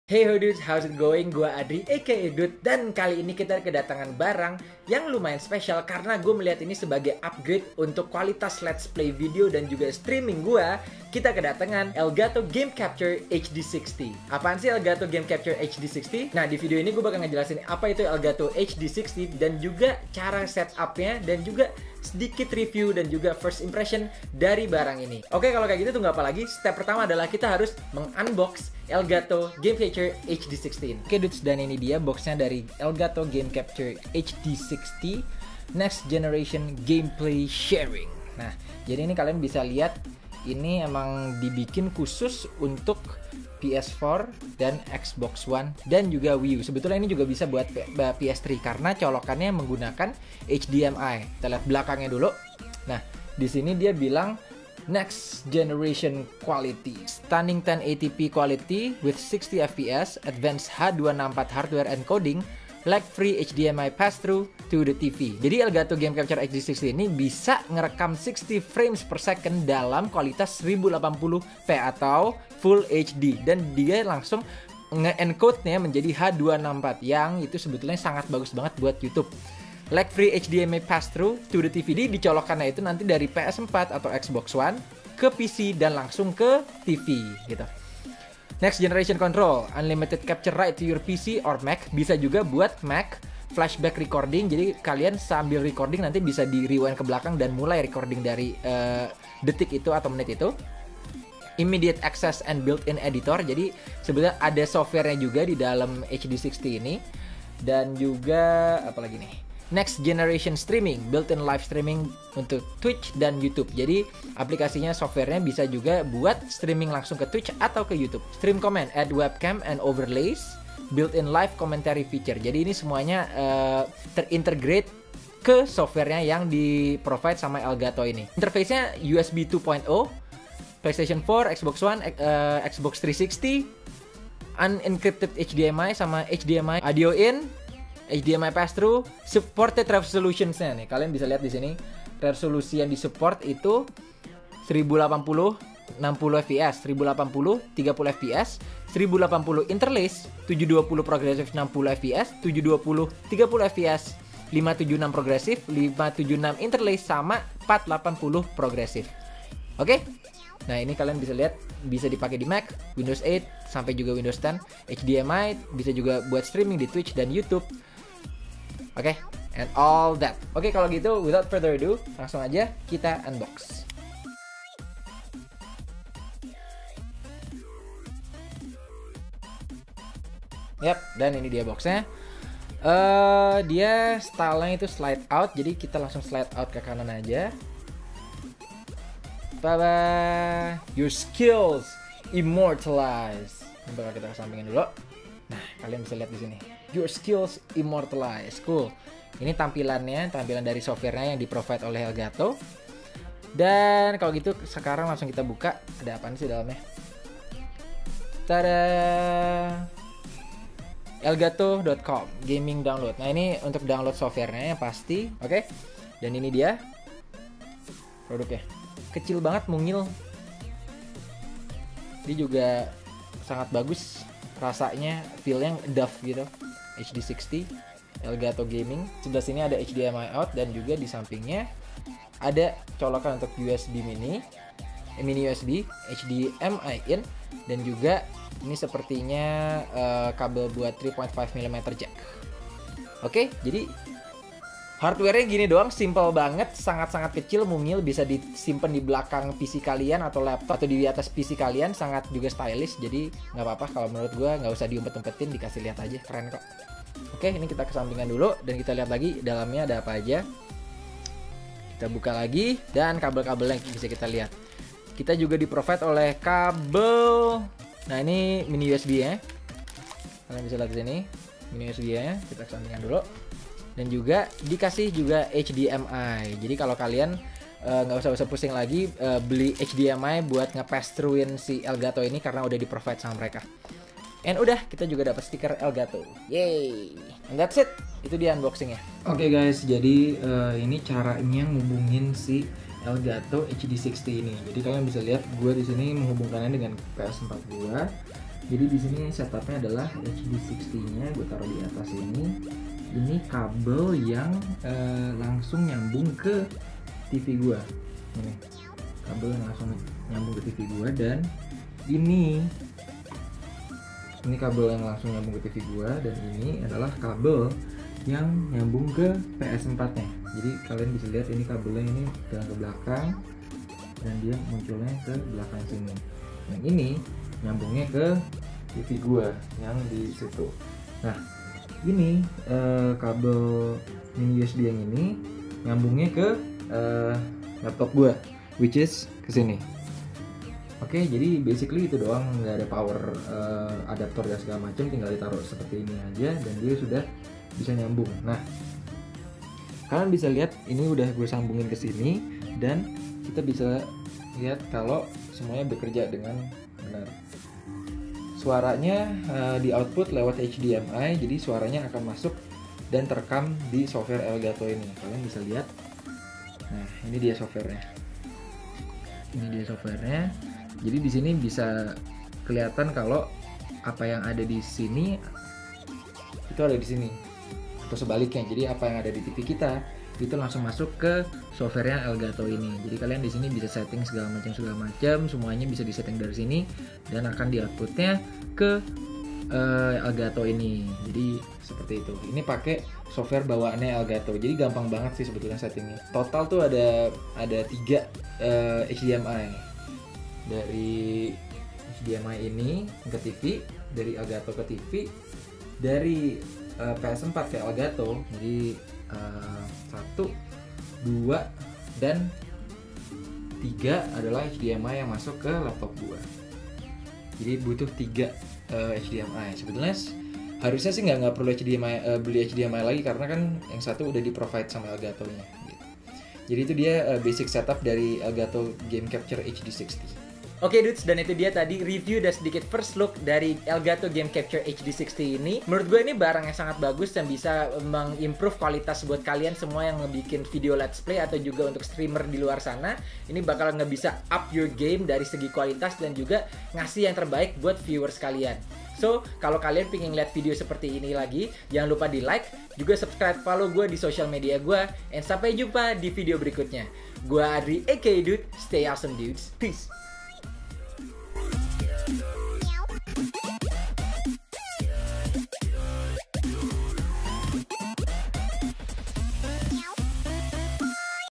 Cara Rekam Game PS4 dan Xbox One kalian dengan Elgato HD60 - Unboxing/Review/How To
Heyho Doods! Satu lagi video unboxing sekalian First look dan how to!

Episode kali ini pas banget nih buat kalian yang mau bikin video Let's Play ala Pewdiepie buat channel Youtube kalian!

Gw bakal ngajarin kalian cara rekam gameplay PS4 atau Xbox One kalian dengan Elgato HD60!

STAY AWESOME DOODS!